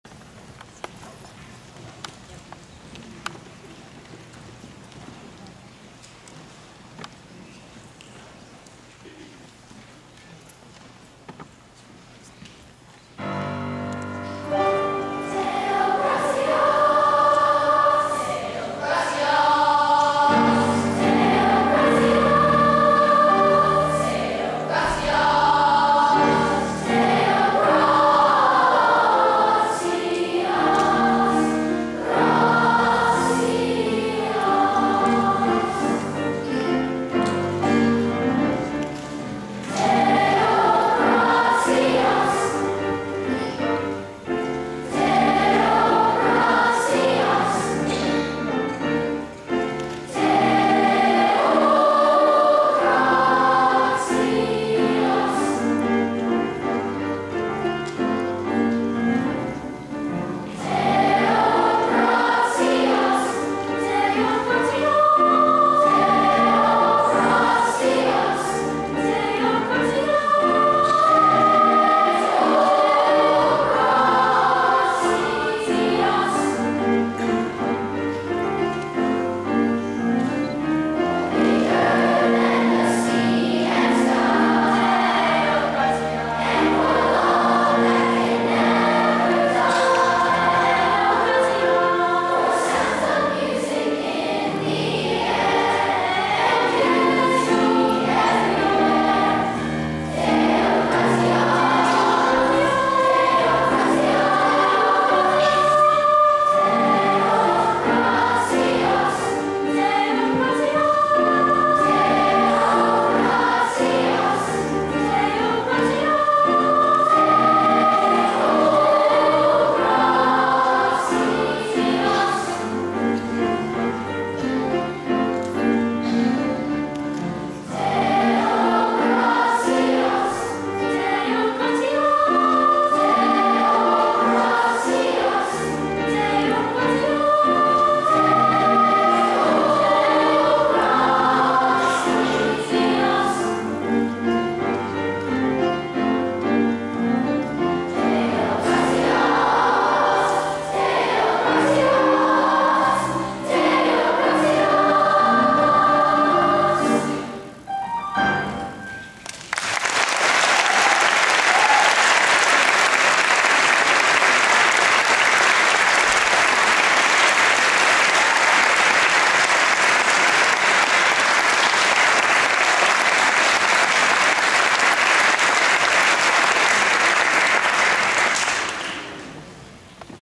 Kelsey's Choir concert
Here is Kelsey's performance at a choir concert.  You may want to forward ahead by about one minute to get to the section where she is featured with a couple other girls.